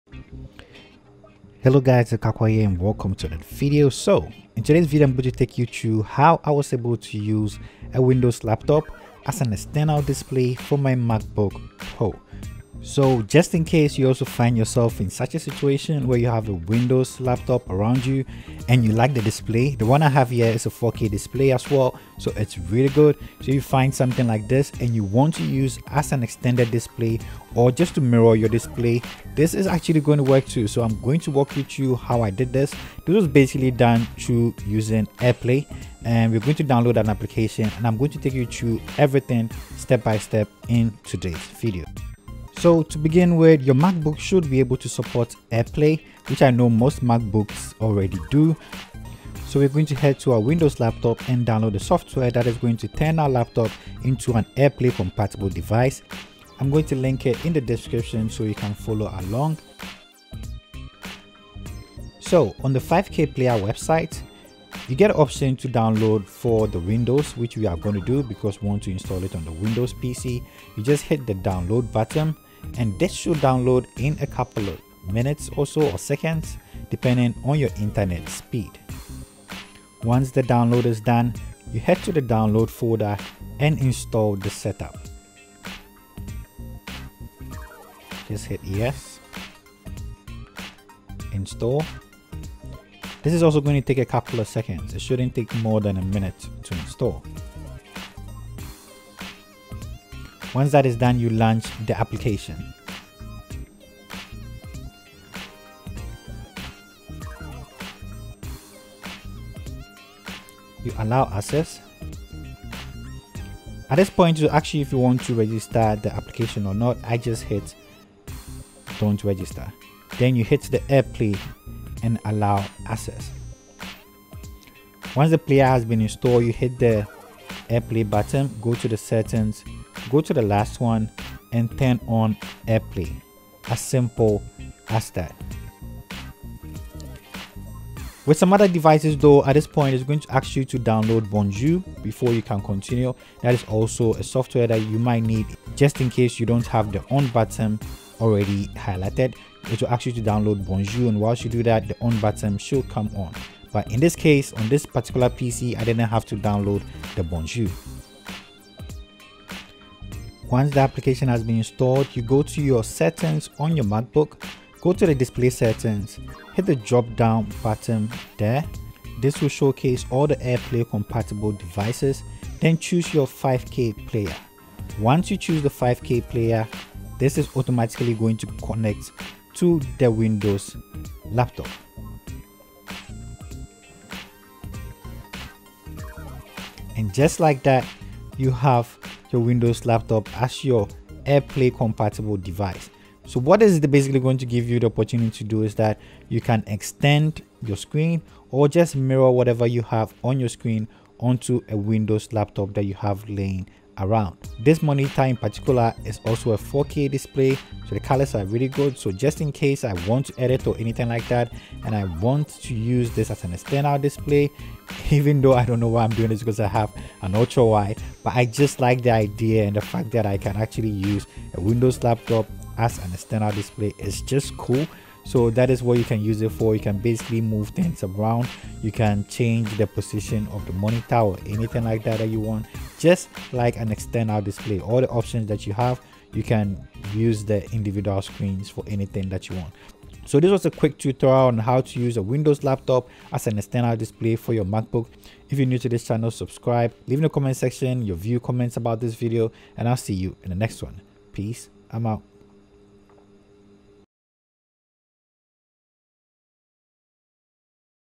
How To Extend Your Macbook Screen To Windows Laptop Airplay
In this video, I am going to share with you How To Extend Your Macbook Screen To a Windows Laptop //  windows laptop as an external for your Mac
this is made possible through airplay
once both computers are on the same network
download and install the 5k player linked below on your windows pc and install

Follow the guidelines I’m the video and easily extends your MacBook screen to your windows laptop
you can easily use display as an  external
display or just mirror ur Mac screen

Editing Laptop - 14 inch Macbook
Main Camera EOS R
Shot on Canon M50
Zoom h4n Recorder
Lens 50mm
Turn table